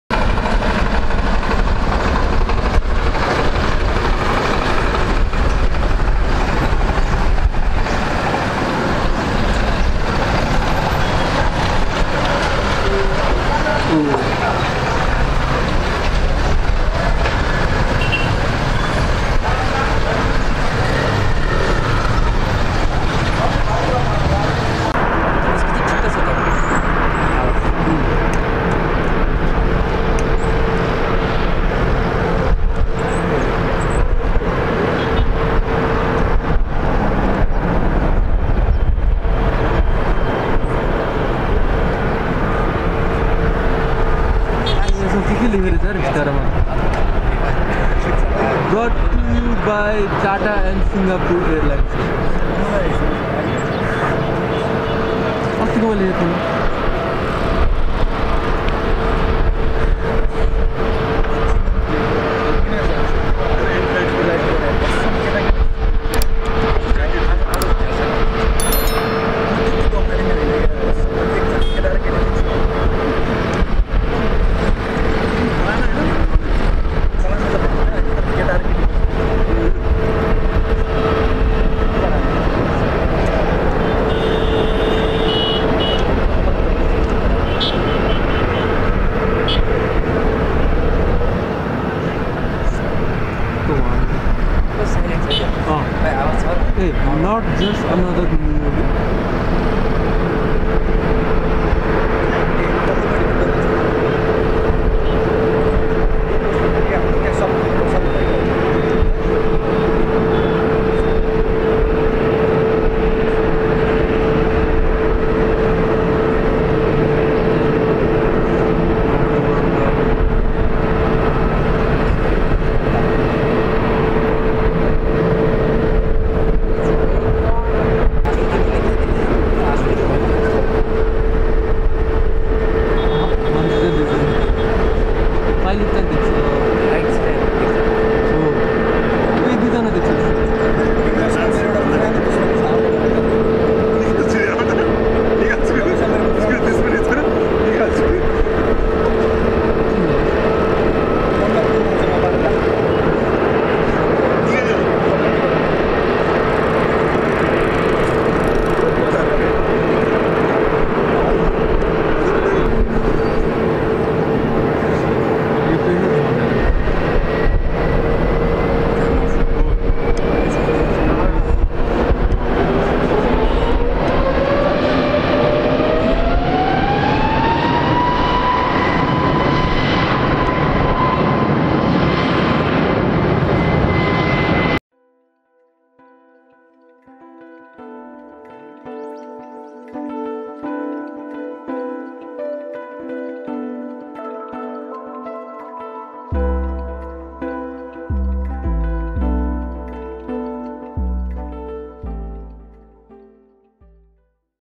Vistara Airlines A320Neo NotJustAnotherNeo at TIA Kathmandu, Nepal. भिस्तारा एयरलाइन्स काठमाडौंमा!
Here in this video we get to spot the very first Airbus A320Neo (VT-TNB) of Vistara, with special livery called hastag NotJustAnotherNeo, at Tribhuvan INternational Airport, Kathmandu, Nepal on April 01, 2022 and March 18, 2022.